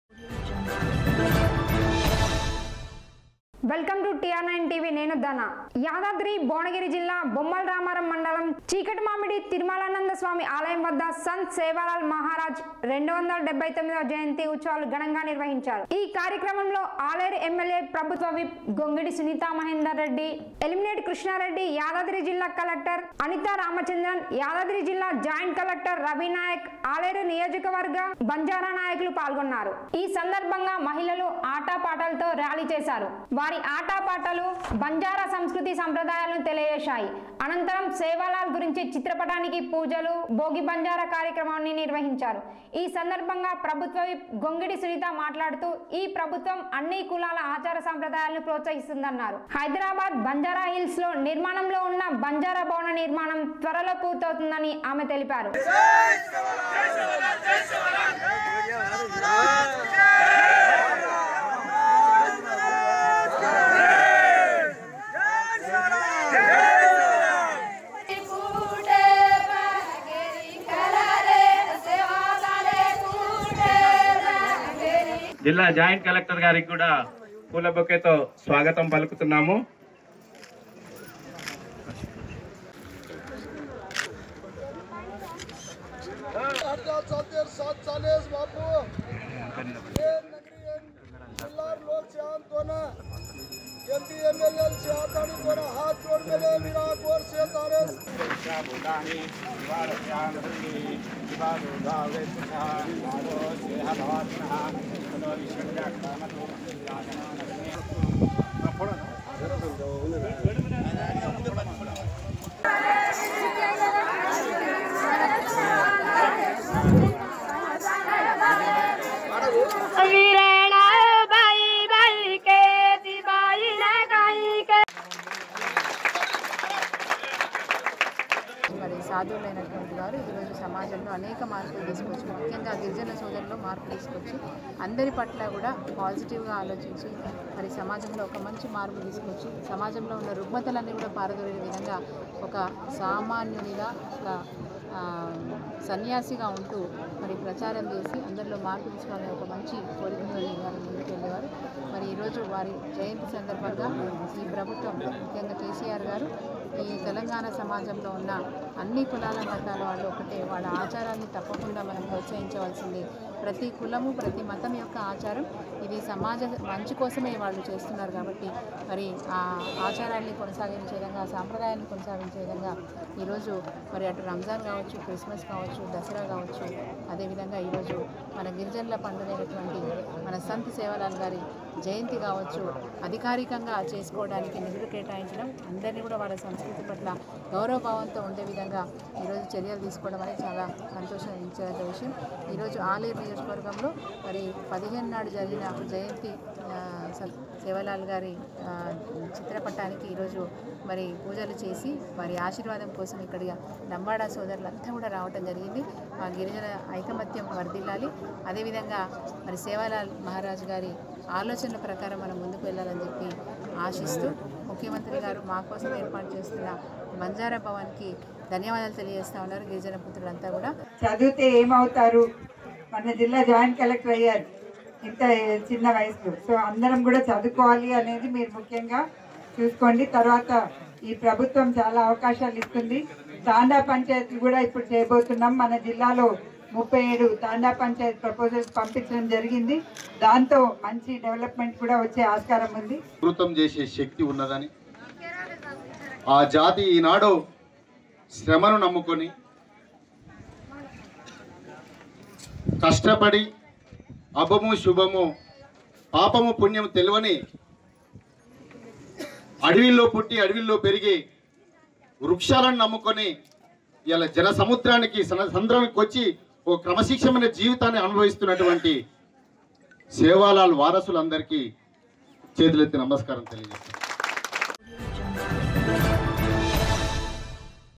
ఘనంగా సంత్ సేవాలాల్ మహారాజ్ 279వ జయంతి ఉత్సవాలు || MLA SUNITHA || Bommalaramaram || TR9TV
ఘనంగా సంత్ సేవాలాల్ మహారాజ్ 279వ జయంతి ఉత్సవాలు || Bommalaramaram || TR9TV